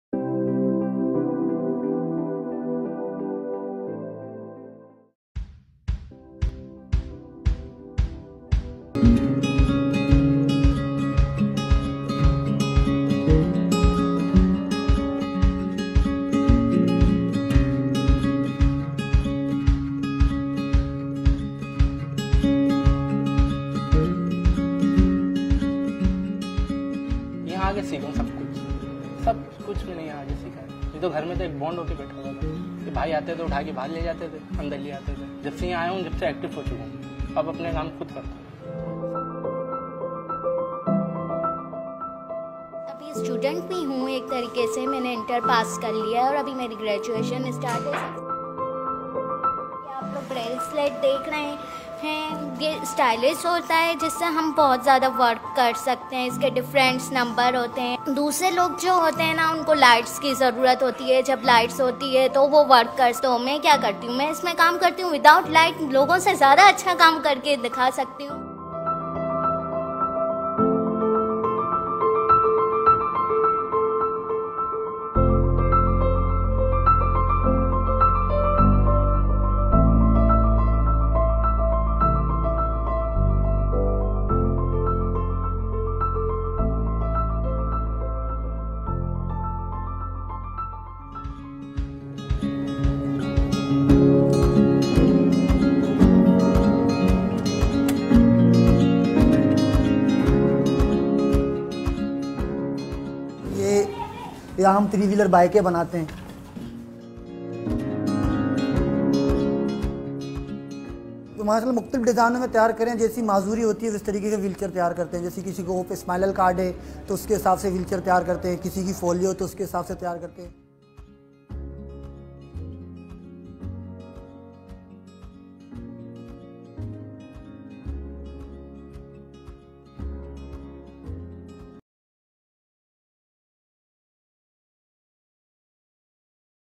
HANDS Independent Living Center
HANDS Independent Living Center with DWA (Disability Welfare Association) has been working for the rights and welfare of PWD’s (Persons with Disabilities) on national and international level. As Pakistan is a signatory member of CPRD (International Convention on the Rights of Persons with Disabilities. The Center is located at Karachi, Sindh